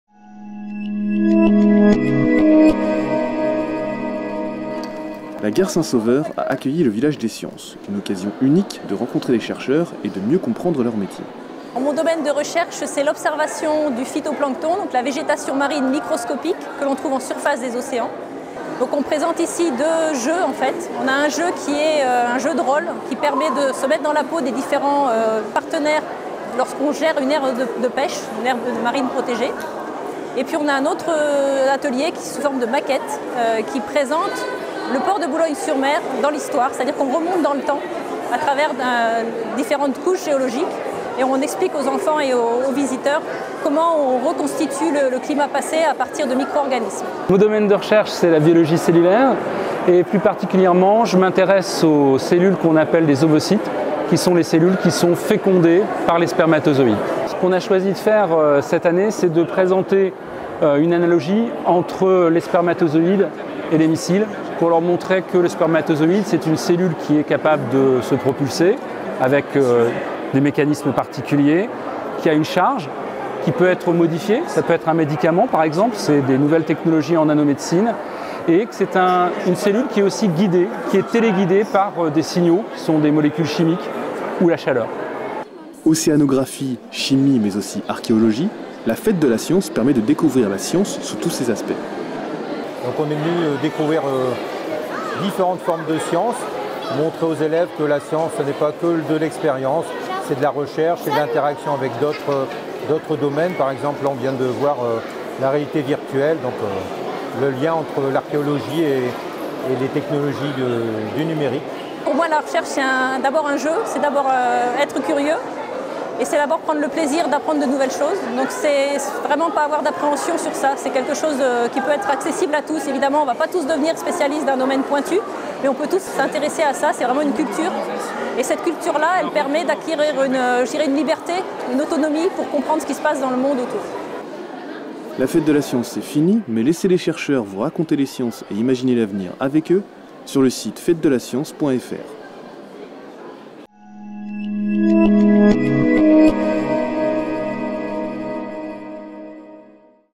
Le village des sciences à saint Sauveur
"Raconter la science, imaginer l'avenir " - Village des sciences de la métropole lilloise 2019

 Vous vous demandez ce que font les scientifiques dans leurs laboratoires et qui ils sont ? Venez à leur rencontre à l’occasion de cette nouvelle édition de la Fête de la science ! Participez à des ateliers et à des jeux, et découvrez des sujets de recherche insoupçonnés. Le thème de « Raconter la science, imagine rl'avenir » sera décliné dans une vingtaine d'ateliers et d‘espaces animés, dans le cadre convivial de la Gare Saint Sauveur à Lille.

Que vous soyez férus de science, néophytes ou simple curieux, en famille ou entre amis, plongez dans la recherche scientifique ! Les activités sont gratuites et accessibles à partir de 11 ans sans prérequis.